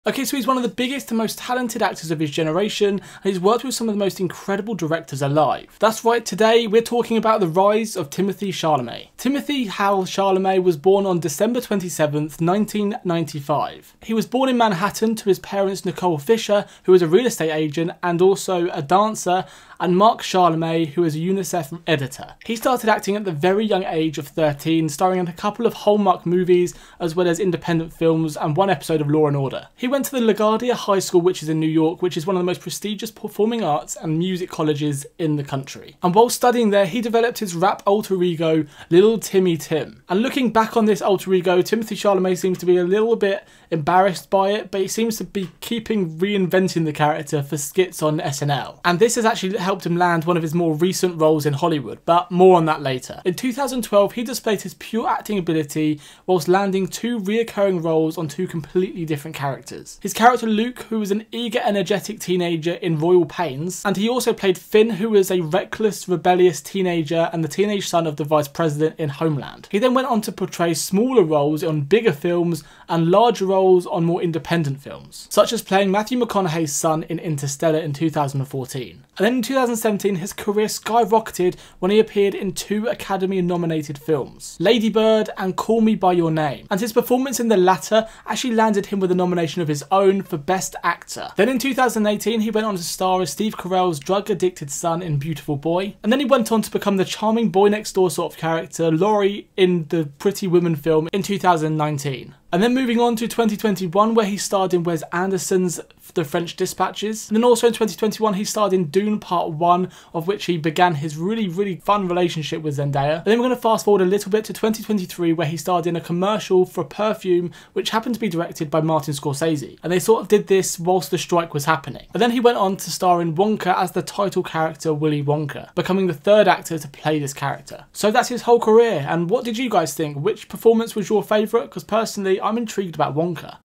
THE RISE OF Timothée Chalamet
In this video, I go over the remarkable rise of Timothee Chalamet from his drama school years to his latest role in Wonka.